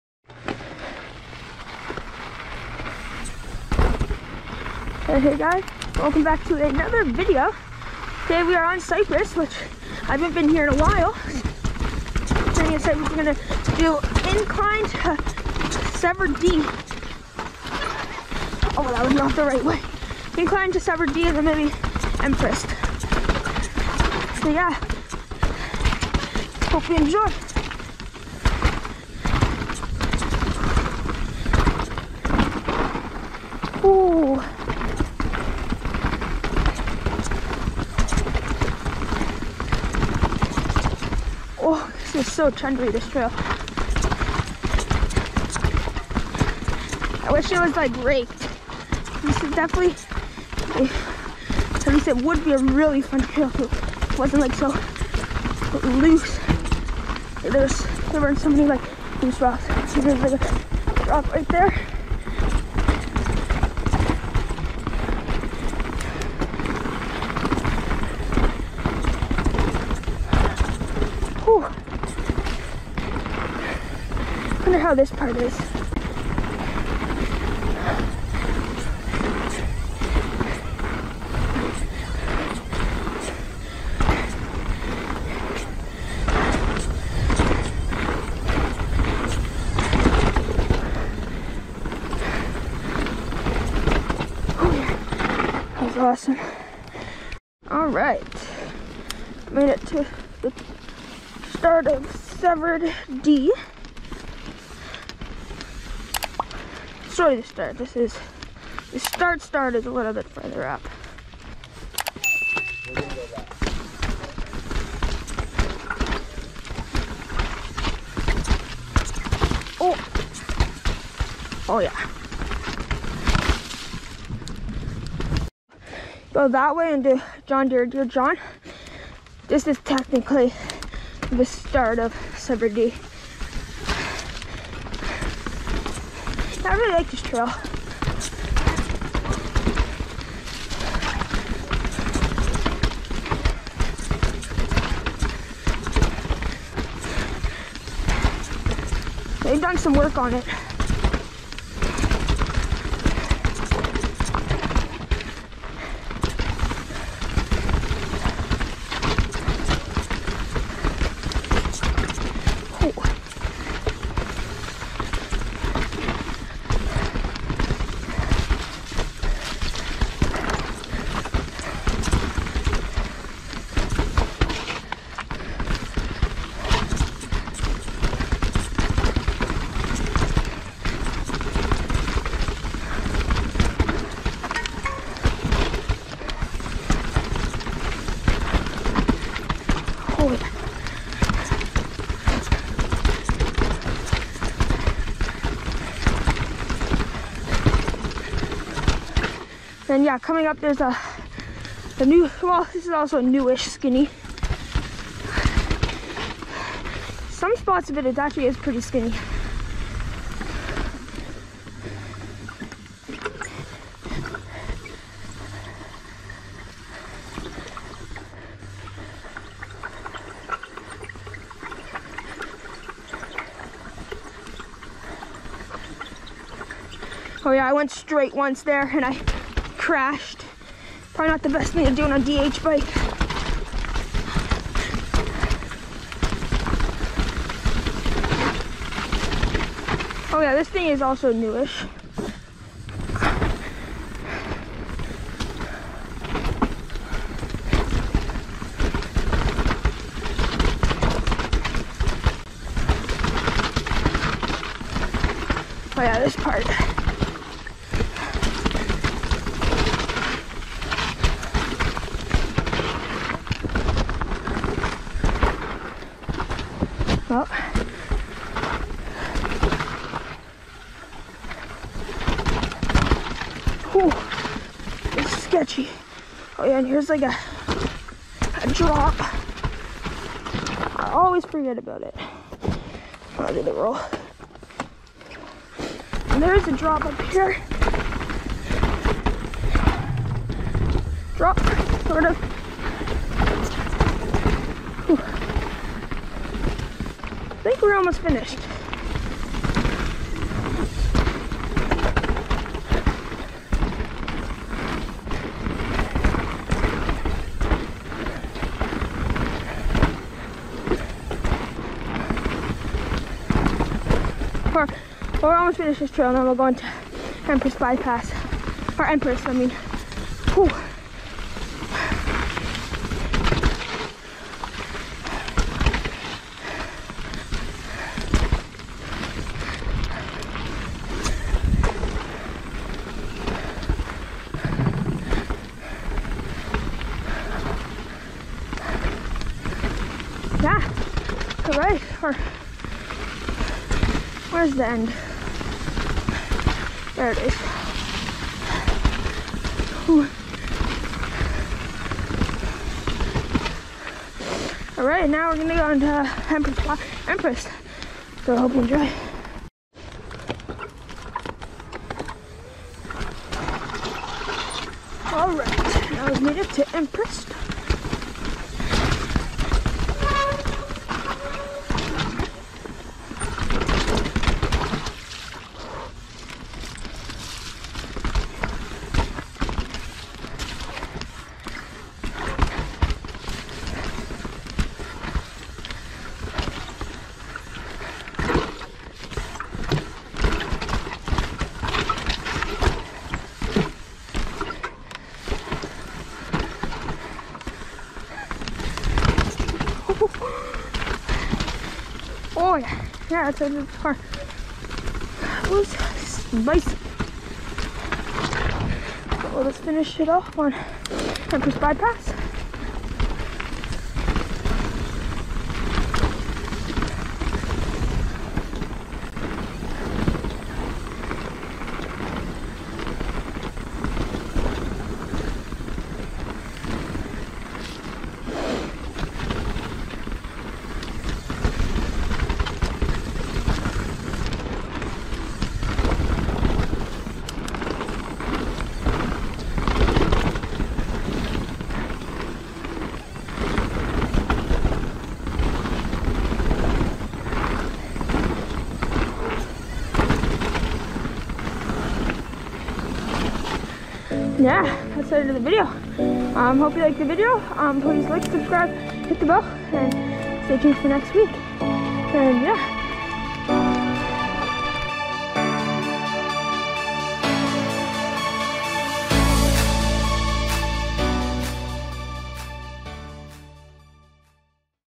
Riding Severed D On Seymour MTN | MTB
MTB Life | Hello and today we are riding a very rough, but fun trail on Seymour. We are riding Severed D. Hope you enjoy.

Trails
Incline 0:00 Black
Power Line 1:07 Blue
Severed D 1:38 Black
Empress 6:00 Double Black
Empress Bypass 6:48 Green
End 7:33